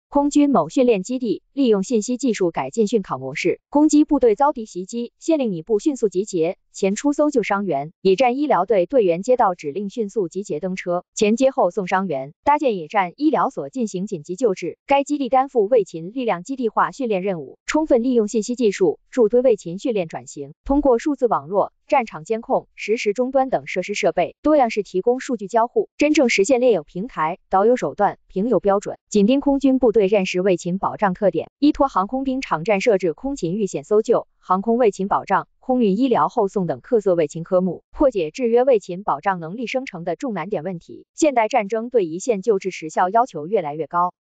空军某训练基地利用信息技术改进训考模式
“攻击部队遭‘敌’袭击，现令你部迅速集结，前出搜救‘伤员’！”野战医疗队队员接到指令迅速集结登车，前接后送“伤员”，搭建野战医疗所进行紧急救治……前不久，来自各战区空军和空军军医大学的9支野战医疗队，集聚空军某训练基地，围绕卫勤组织指挥、伤员分类后送、战伤紧急救治等多个课题展开演练，在真训实练中锤炼战时卫勤保障能力。$$$该基地担负卫勤力量基地化训练任务，充分利用信息技术助推卫勤训练转型，通过数字网络、战场监控、实时终端等设施设备，多样式提供数据交互，真正实现练有平台、导有手段、评有标准；紧盯空军部队战时卫勤保障特点，依托航空兵场站设置空勤遇险搜救、航空卫勤保障、空运医疗后送等特色卫勤科目，破解制约卫勤保障能力生成的重难点问题。$$$“现代战争对一线救治时效要求越来越高。”该基地主任、演练总导演李鑫说，近年来基地聚焦战训一致新要求、按纲施训新标准，先后轮训30余支野战医疗队。他们围绕体系标准、能力评估、规范流程等关键环节，完善改进训考模式，进一步提升了参训分队战备建设水平、作业运行效率。（杨峰、余伟）$$$$$$